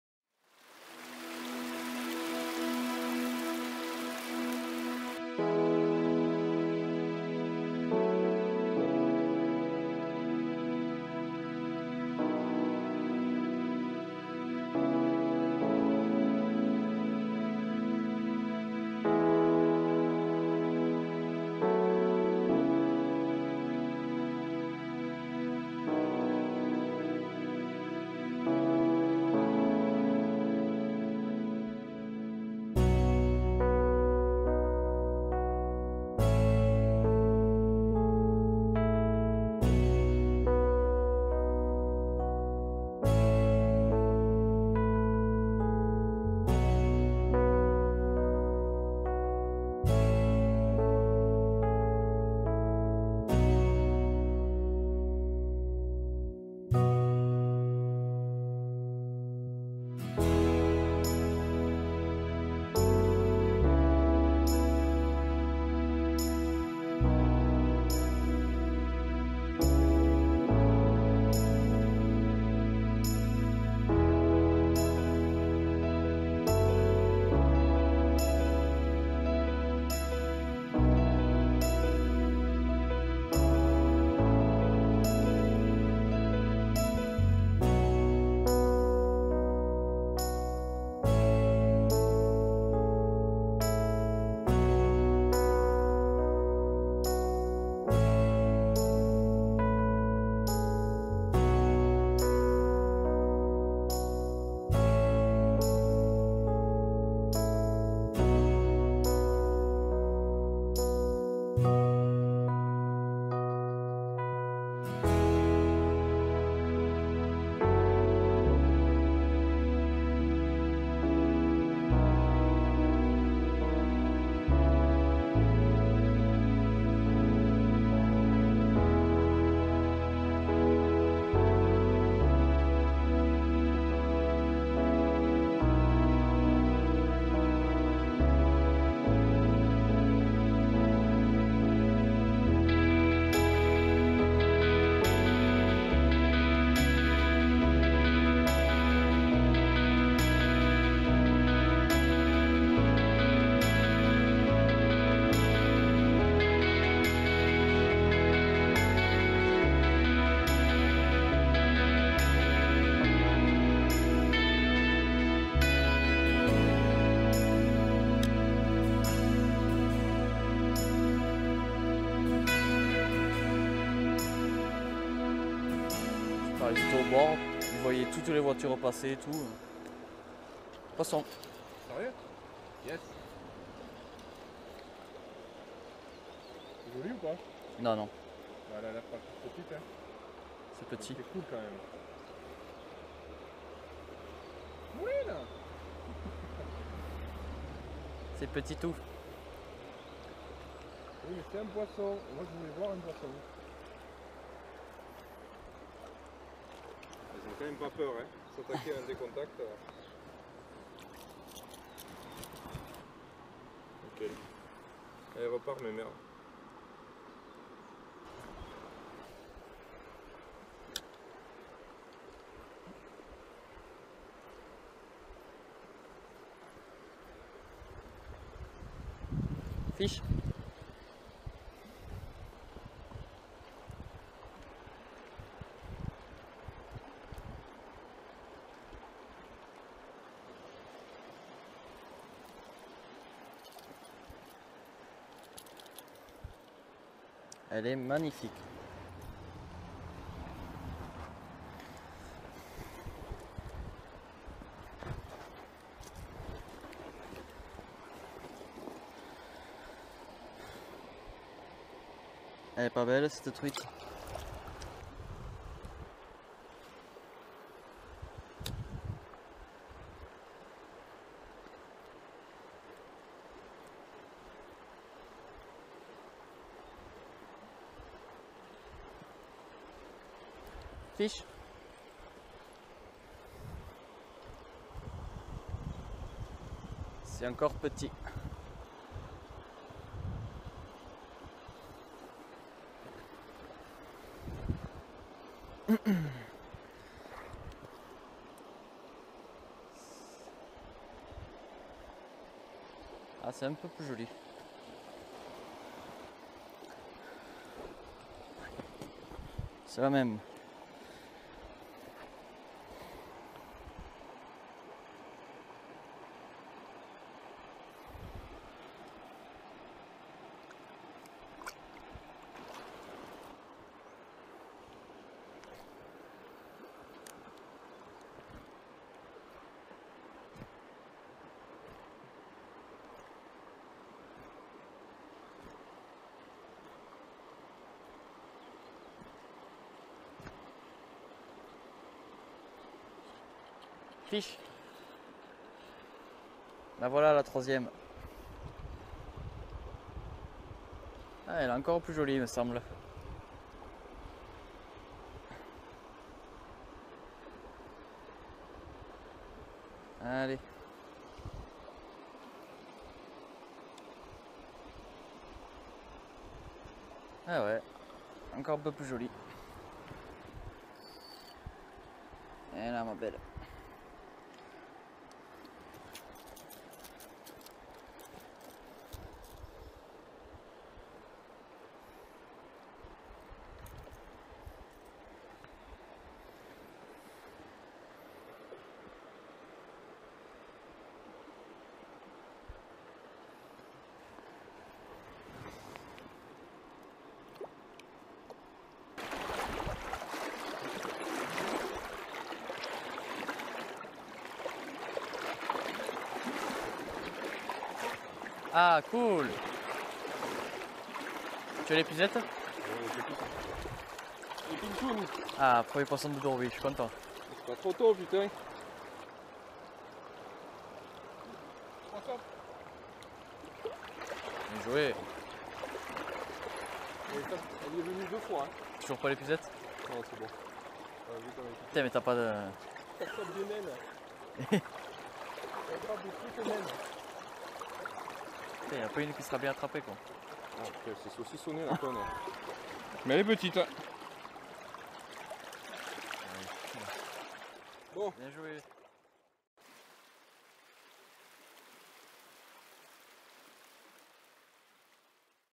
Truites aux leurres - Les joyaux de la Dourbie (feat Julo)
Cette fois ci, on est au milieu de la Dourbie, Julo n'attend qu'une chose: prendre une truite de cette magnifique rivière aux eaux limpides. Après une matinée de pêche sous un grand soleil, la mission est enfin achevée.

On se retrouve bientôt pour une nouvelle vidéo. :)